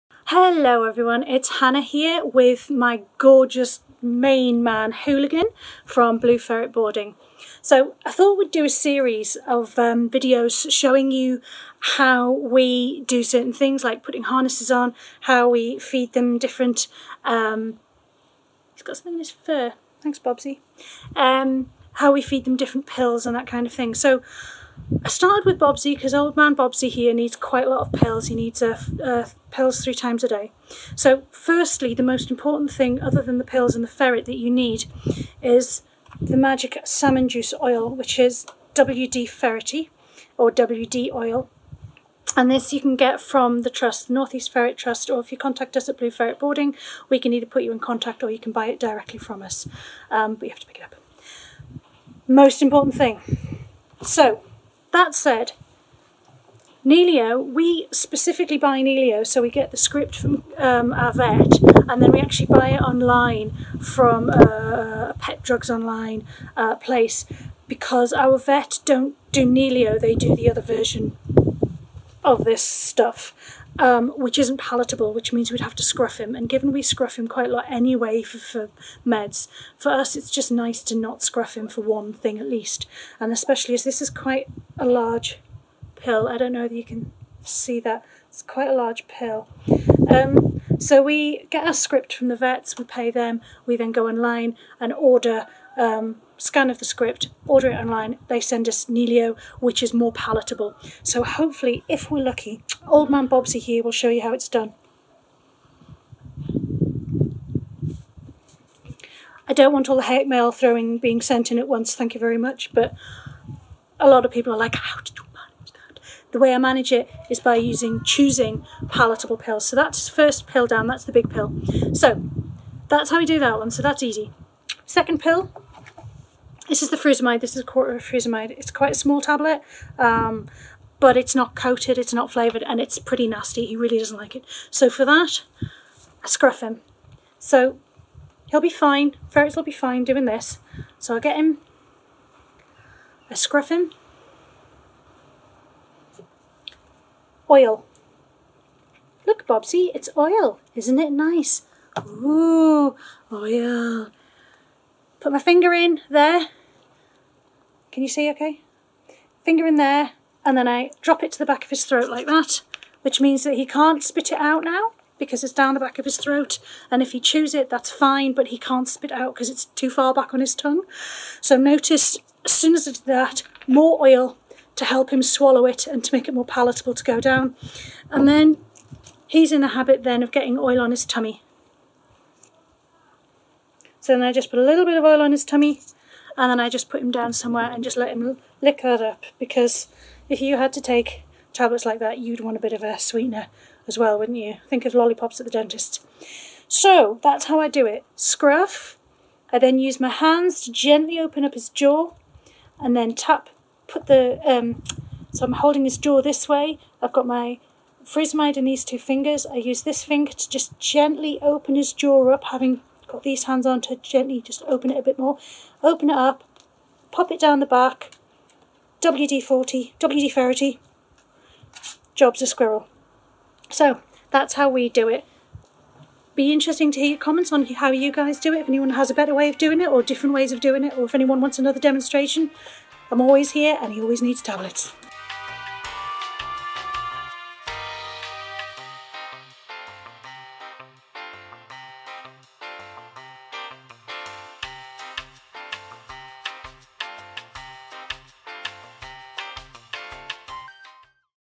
How To Give a Ferret Pills
In this video Hannah from Blue Ferret Boarding will show you
how to feed medication to your ferret. The wonderful Hooligan
has assisted Hannah in showing you how we get pills into your
ferret and the things you can do to make it as easy as possible.

This video is the first in a series where we will cover some of
the things you need to be aware of when taking care of ferrets.

for a selection of ferret items and our hand made harnesses.